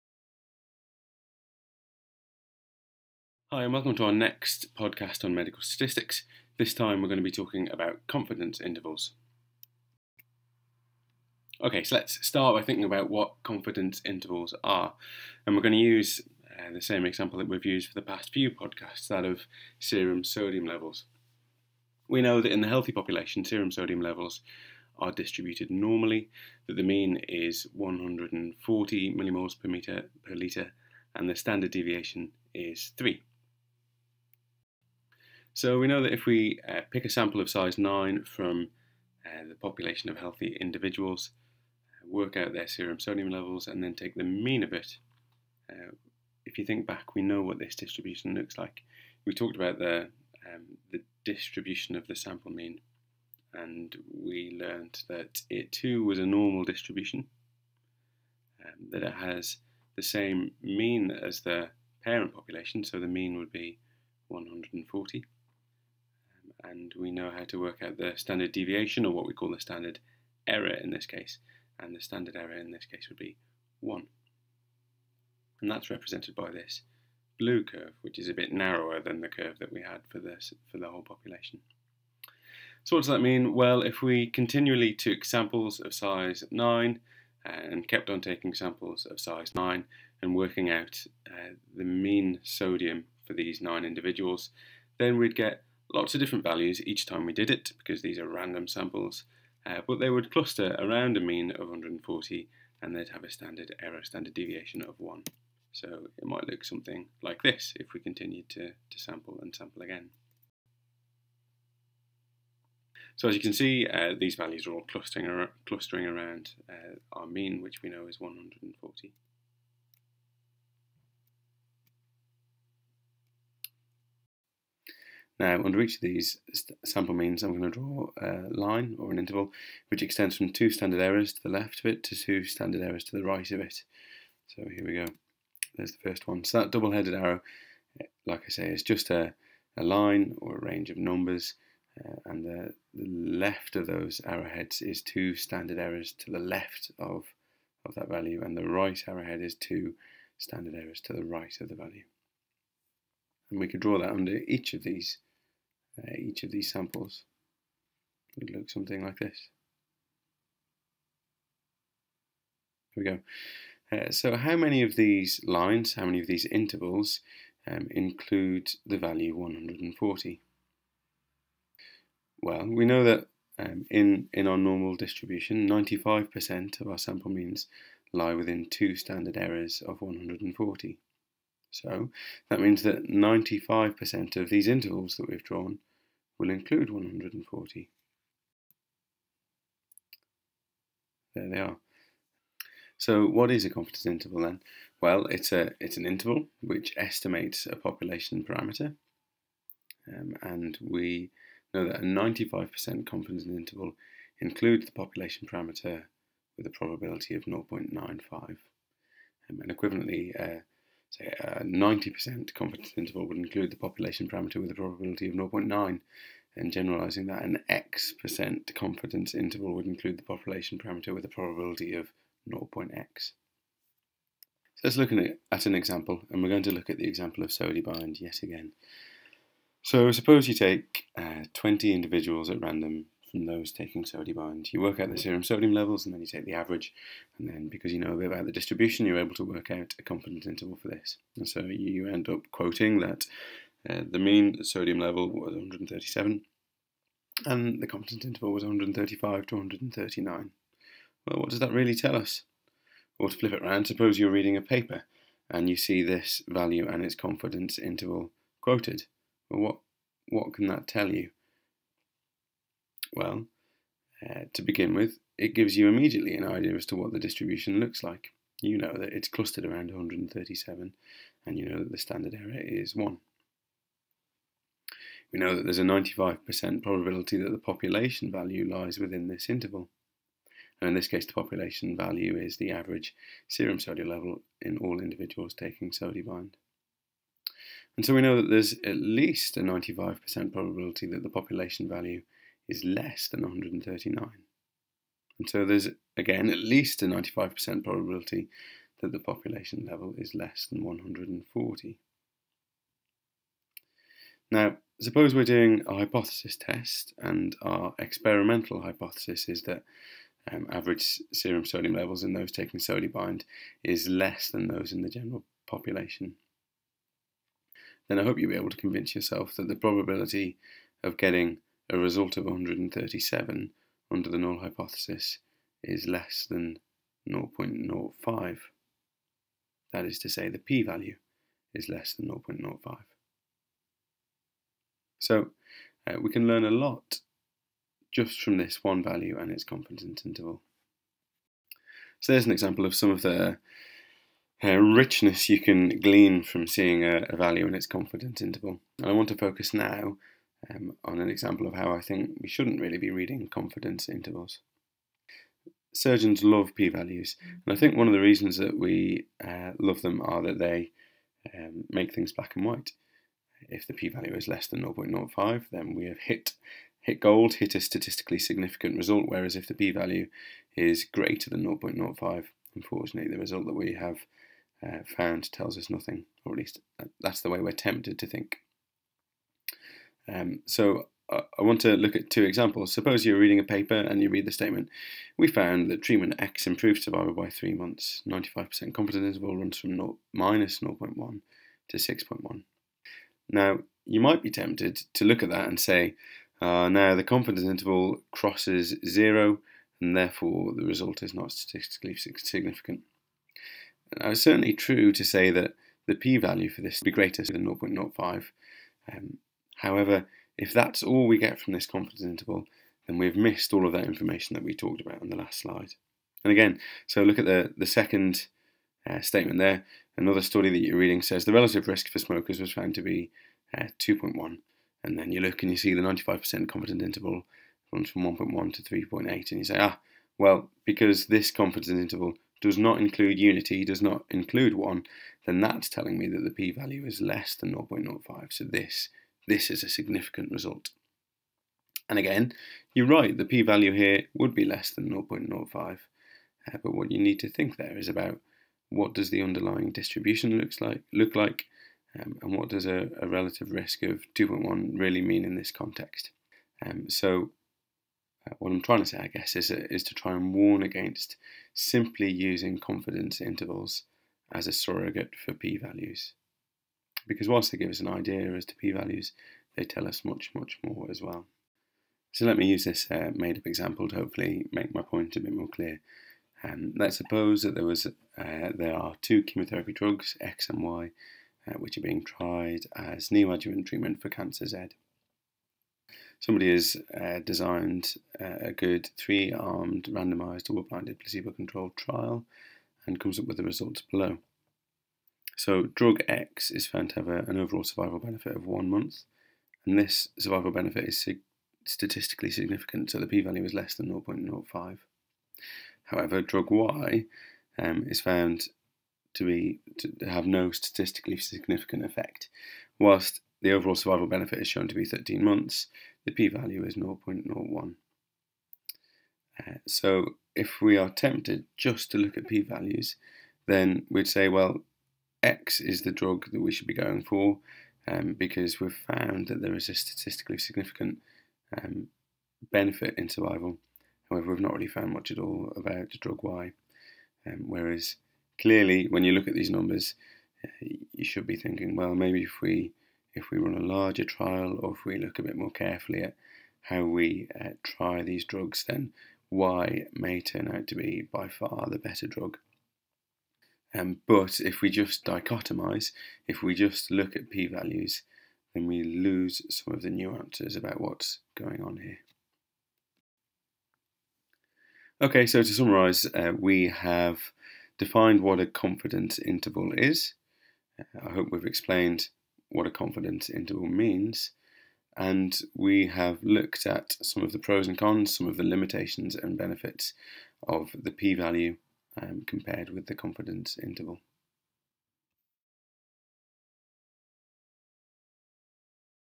Medical Statistics VII: confidence intervals
How confident are you with confidence intervals? Perhaps not 95%.
In this podcast Rob Radcliffe explains what confidence intervals are, how to interpret them and how they provide a deeper understanding of data and the p value. Essential viewing for any student, trainee or consultant/attending reading a scientific paper and wanting to interpret the data correctly.
Rob Radcliffe is a former maths teacher and is now a trainee in Urology in the East Midlands, UK